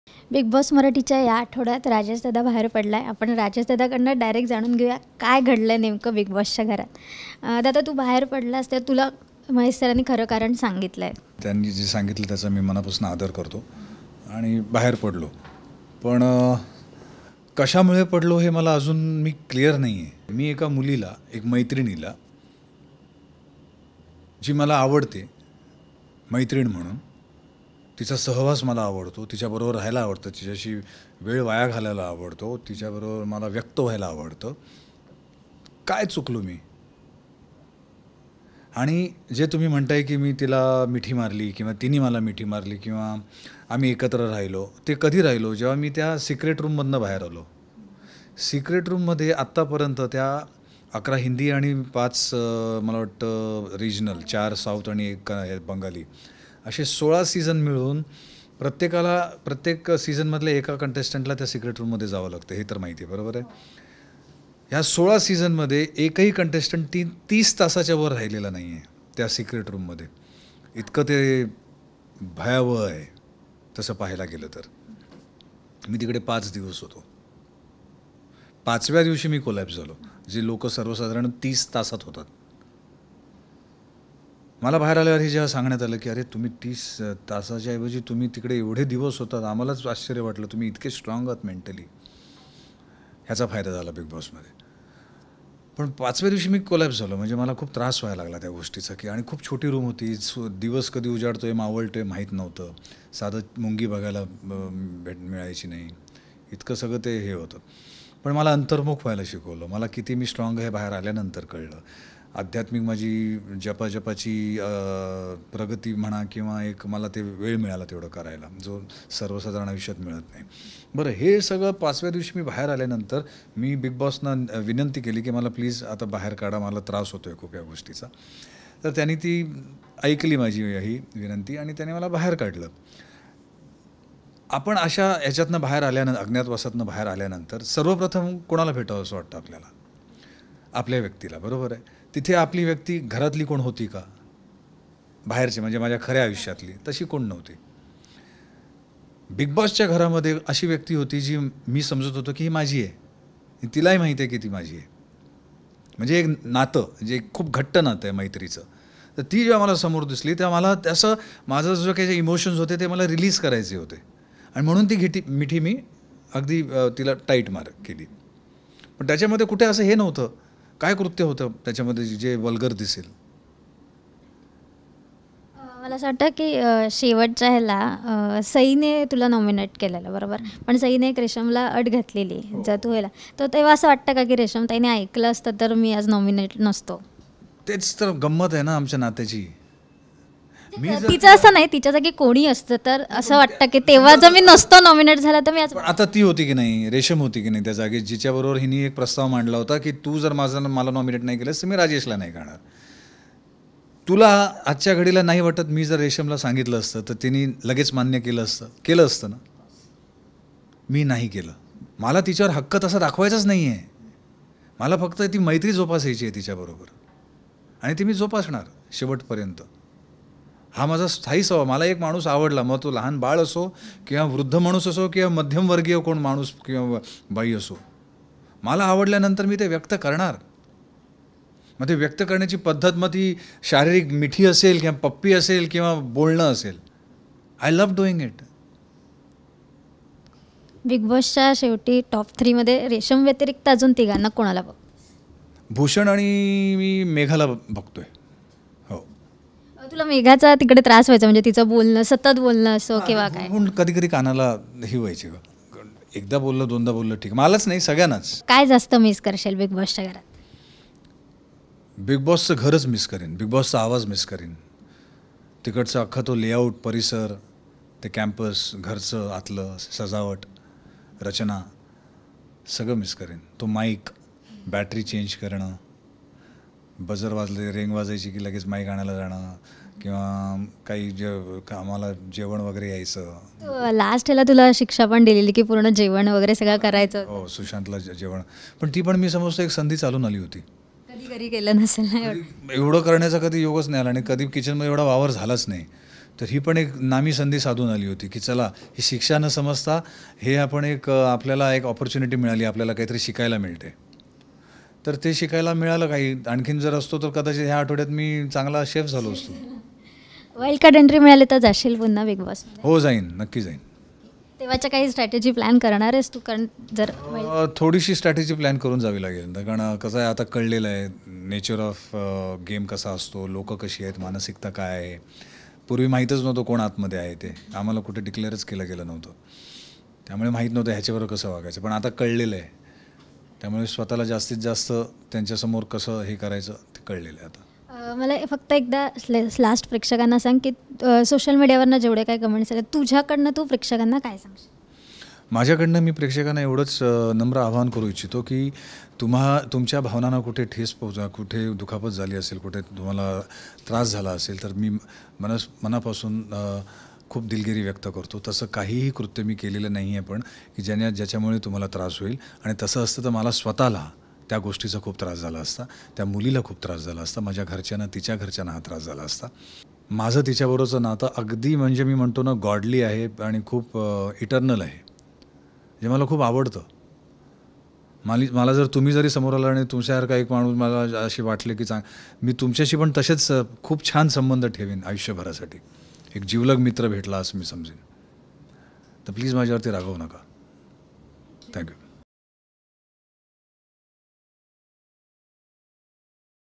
Exclusive Interview With Rajesh Shringarpure | Bigg Boss Marathi | Colors Marathi | Mumbai Live
In an Exclusive Interview with Mumbai Live, the recently ELIMINATED contestant from Bigg Boss Marathi Rajesh Shringarpure talks about his journey in the house,  also shares his take on Resham Tipnis and his relationship.